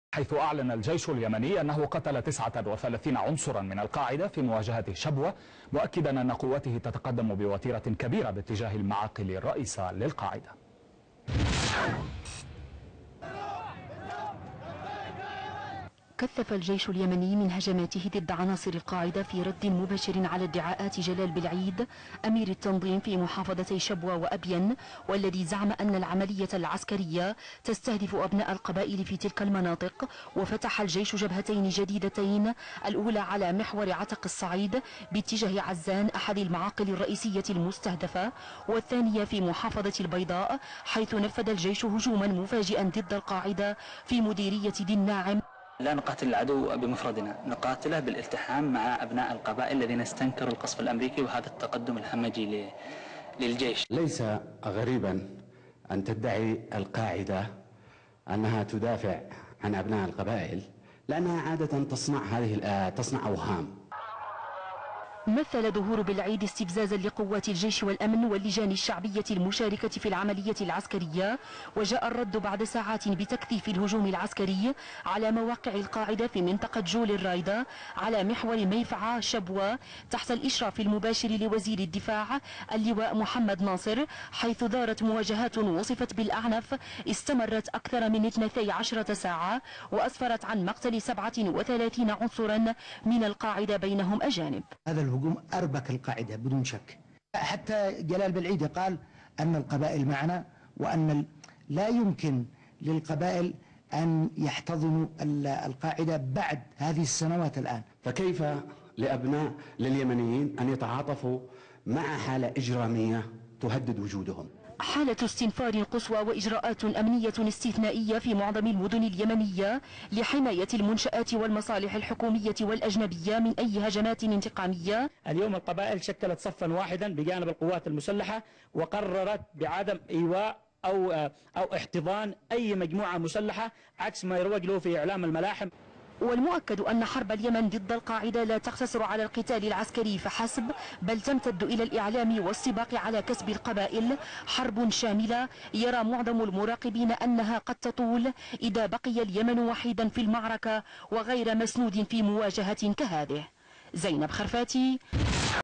الجيش يتقدم نحو معاقل القاعدة في عزان والمحفد ومقتل 37 من القاعدة بشبوة
الصفحة الثانية لثورة التغيير في اليمن